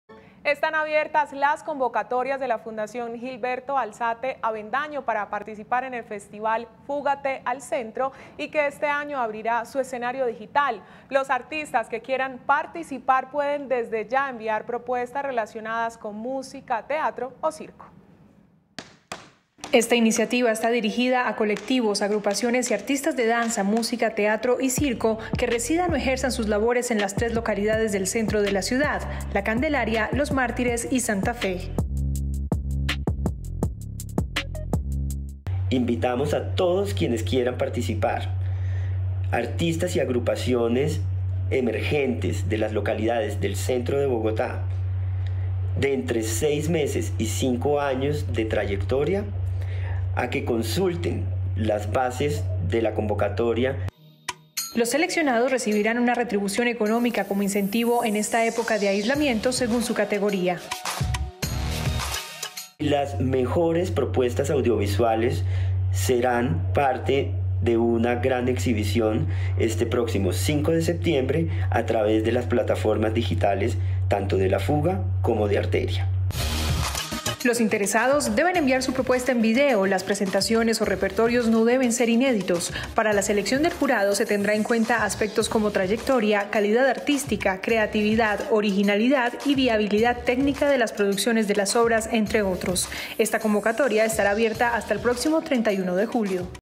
Convocatorias para el festival "Fúgate al Centro"
NoticiasCapital | Esta convocatoria está dirigida a todos los creadores artísticos que vivan en las localidades del centro de Bogotá.